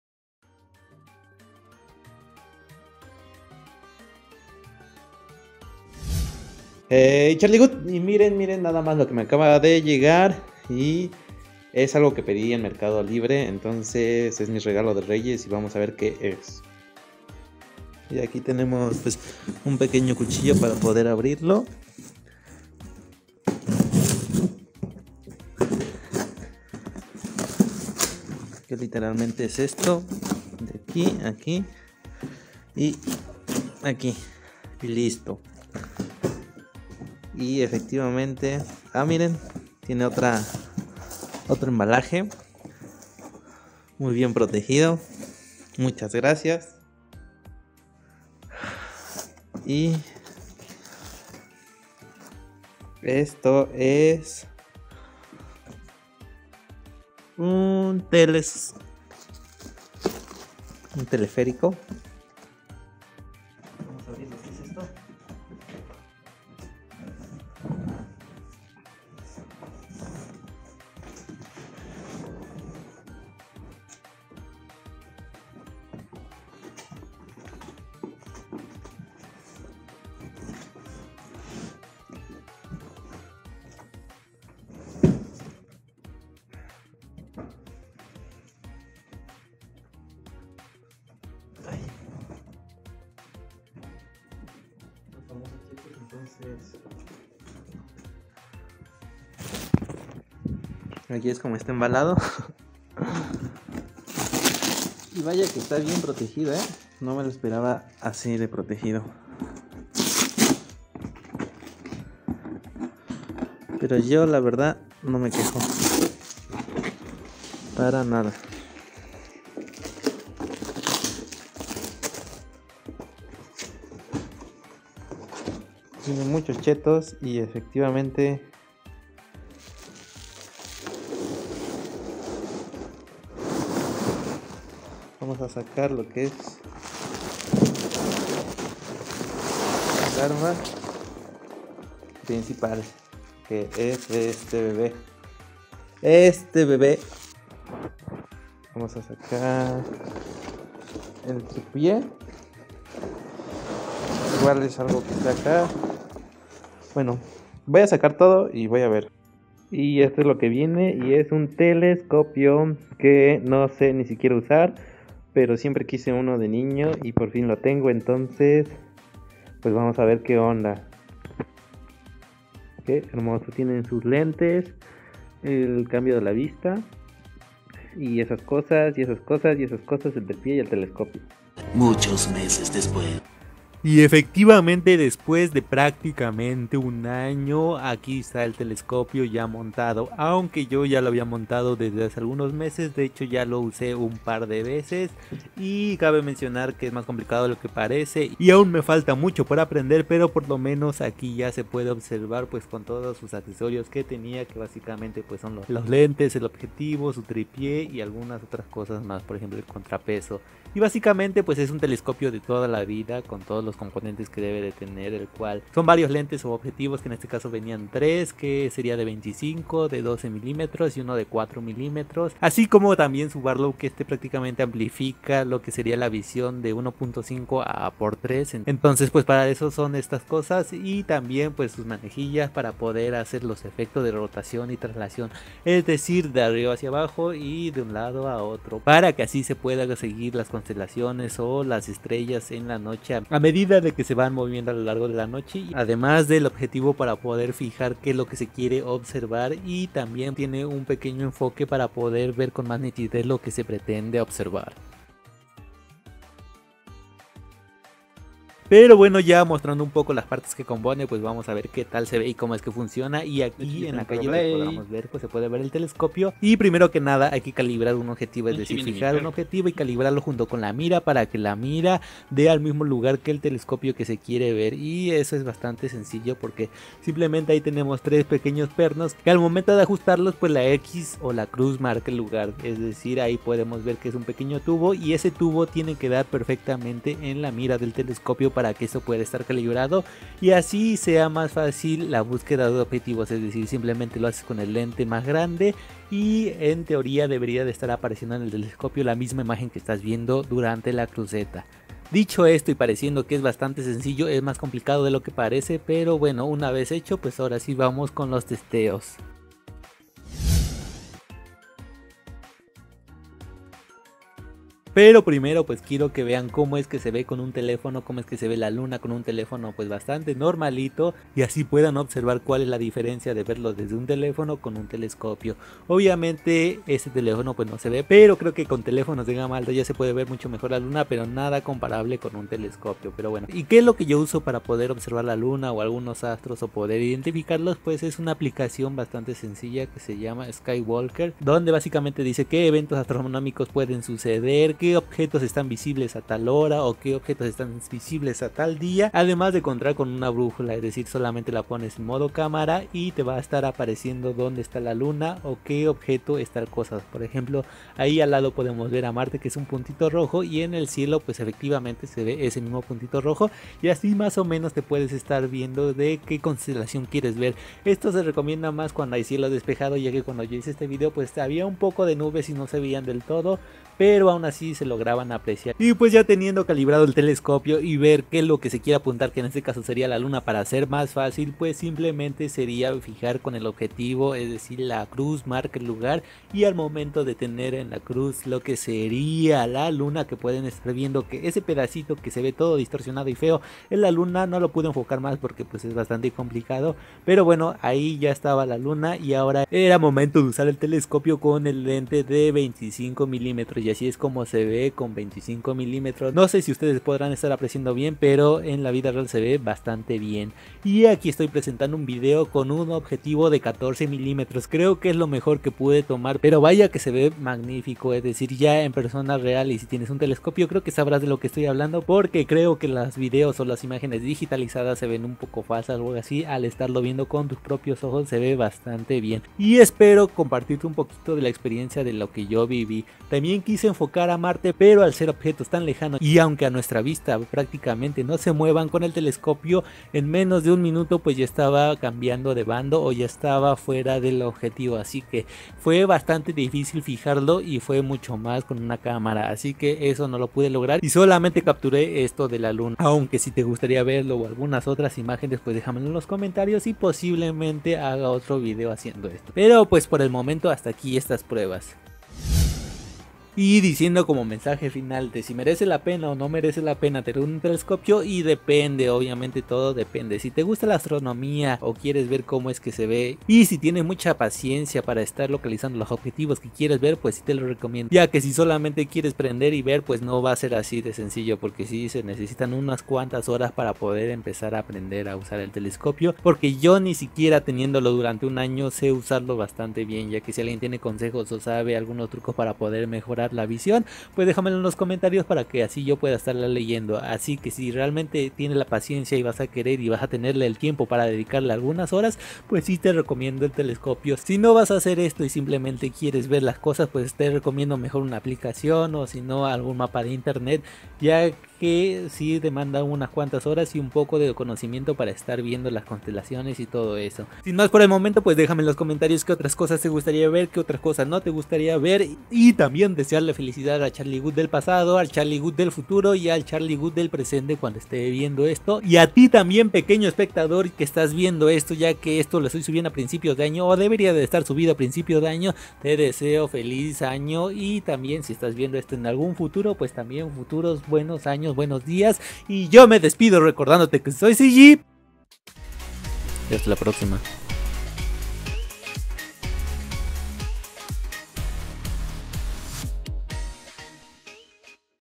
Cómo usar, ver, tocar y sentir un telescopio. Vale la pena?
Y dime tienes un telescopio o algun consejo que darme, te sirvió este video, te entretuvo, te enojo, te estafo dejamelo saber en los comentarios.
Sin mas que decir feliz año a todos y felices tardes si lo ves en otro día del año. besos en el nudo de globo.